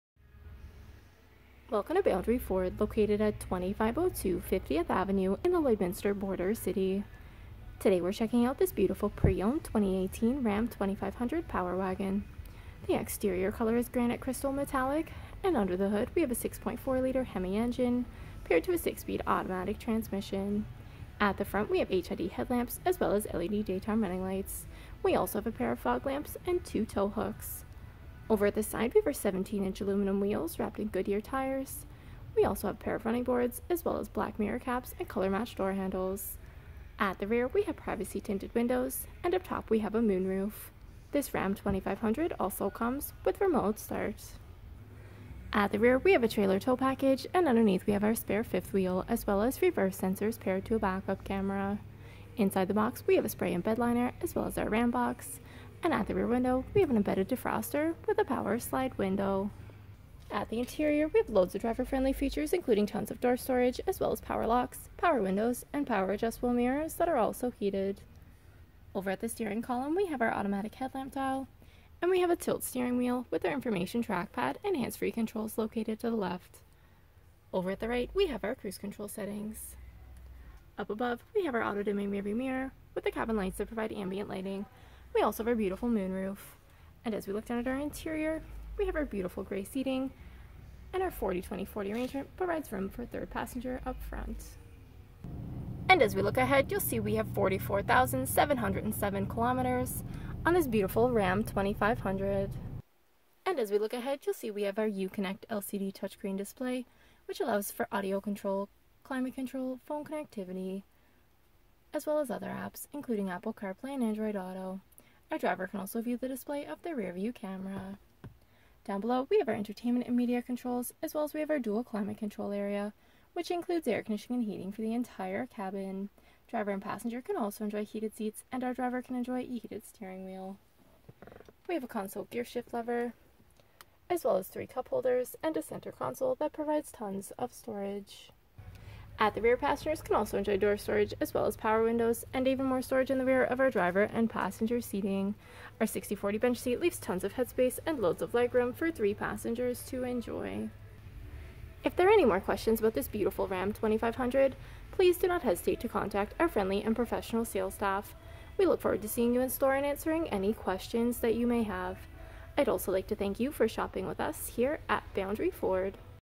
Pre-owned 2018 Ram 2500 Power Wagon W/ 6.4L HEMI Moonroof Overview | Boundary Ford 19T313A
Welcome to the pre-owned 2018 RAM 2500 Power Wagon! Powered by a 6.4L HEMI engine paired to a 6 speed automatic transmission with 4 wheel drive and only 44,438 KMS on the odometer! The beautiful Granite Crystal Metallic exterior is carried throughout and complimented by 17 inch aluminum wheels. Loaded with incredible features before you even climb inside, including trailer tow package, reverse sensors paired to a backup camera, spray-in bedliner, LED bed lighting and remote start. The incredible features continue at the interior and include beautiful seating for 6, heated front seats, heated steering wheel, moonroof and SO much more! The features are endless, so stop in today to experience everything the 2018 RAM 2500 has to offer! Here at Boundary Ford we're prairie proud and we offer the best deals in the prairies!
STOCK# 19T313A